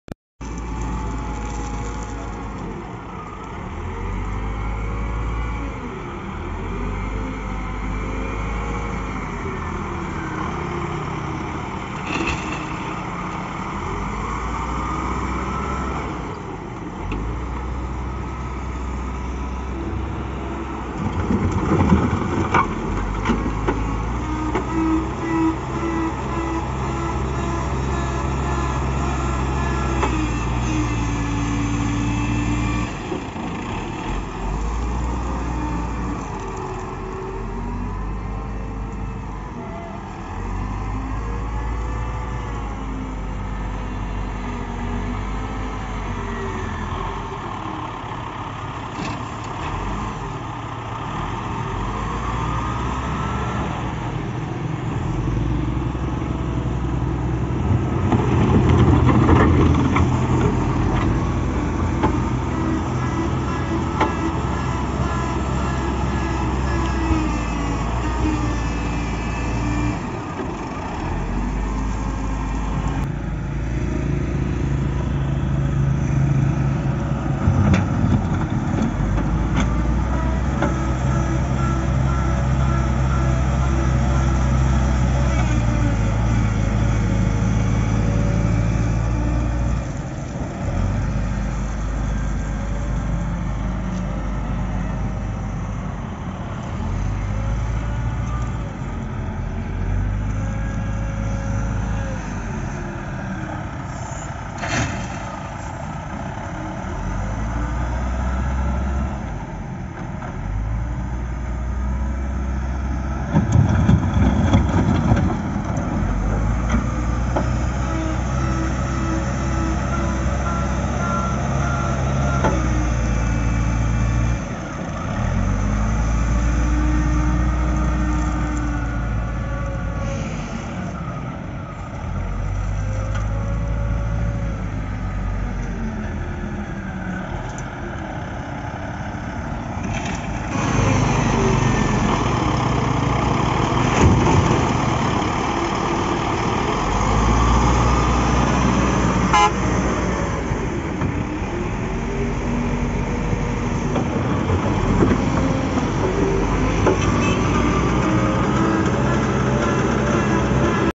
Cat 950B wheel loader agerigat loading a dumpers
caterpillar wheel loader 950B agerigat loading in a dumpers